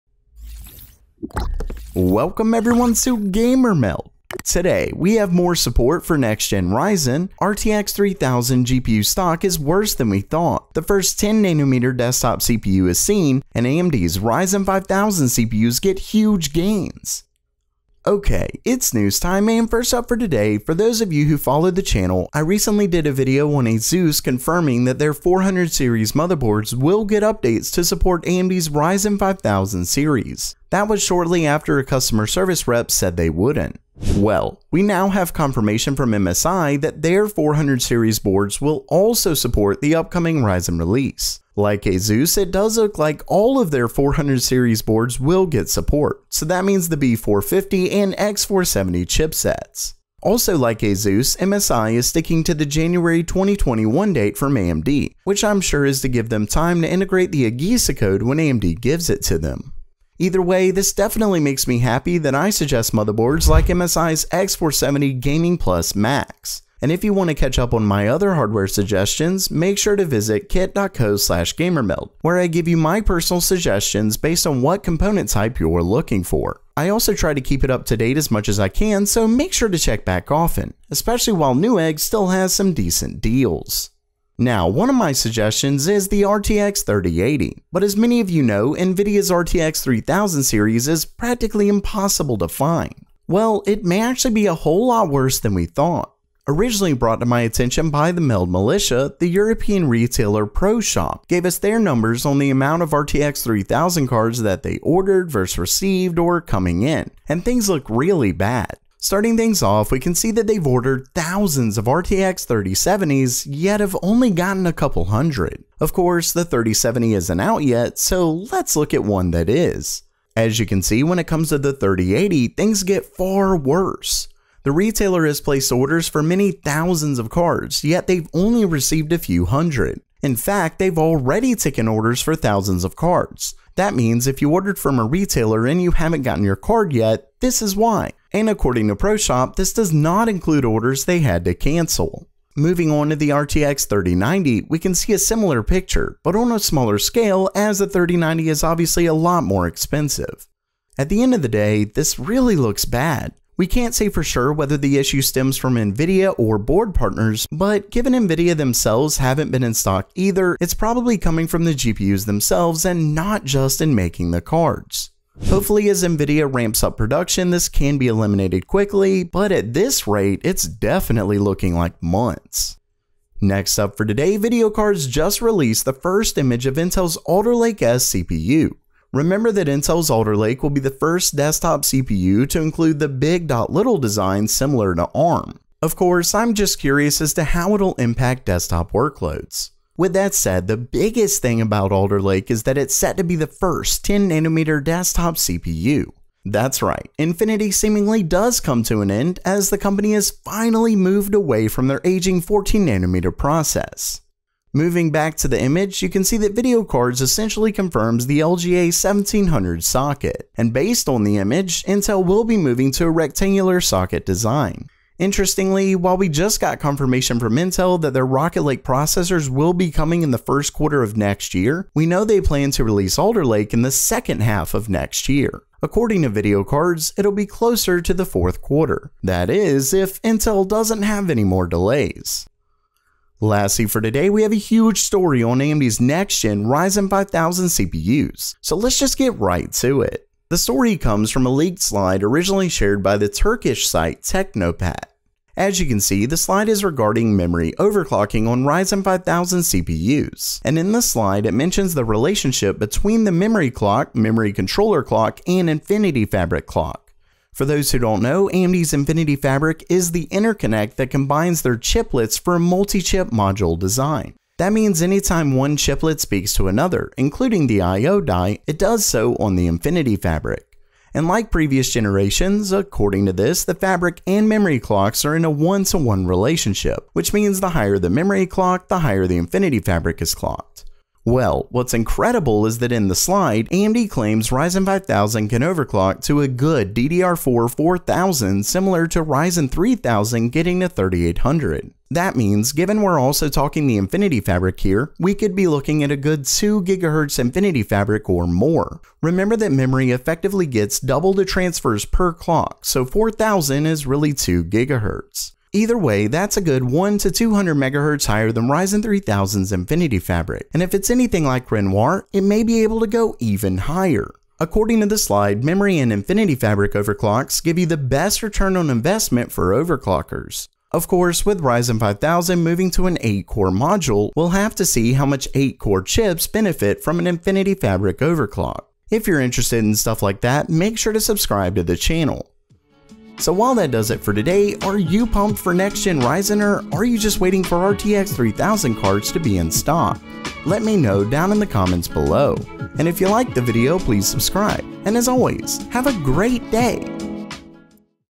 Ryzen 5000 Is WAY More Powerful Than We Thought!
AMD's Ryzen 5000 CPUs can get incredible overclocks, Intel's first 10nm desktop CPU was spotted, RTX 3000 stock is worse than we thought and more motherboards support Ryzen 5000!

Find the best prices & best selection at Micro Center (Sponsored Link)

TIMESTAMPS
0:00 Intro
0:18 More Ryzen 5000 MOBO support
1:24 RTX 3000 Stock Update
2:53 First Intel 10nm CPU Pictured
3:52 Ryzen 5000 Memory Overclocks